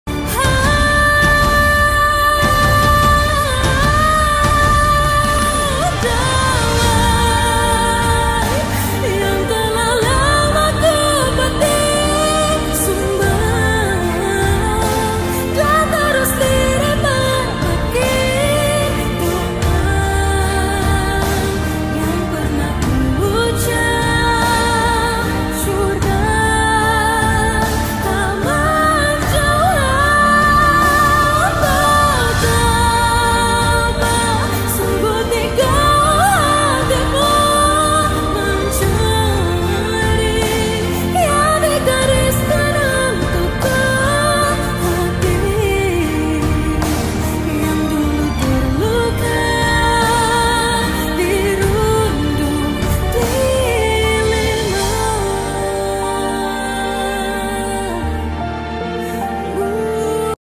Asap Tebal Menyelimuti Bali 14 Oktober 2023 Akibat Kebakaran di TPA Suwung, Denpasar-Bali
Seperti akan masuk ke “negeri di atas awan”, tapi ini adalah asap dari kebakaran TPA Suwung, Denpasar-Bali di area Jl. By Pass Ngurah Rai arah ke perempatan Pesanggaran 14 Oktober 2023 jam 12:38 Wita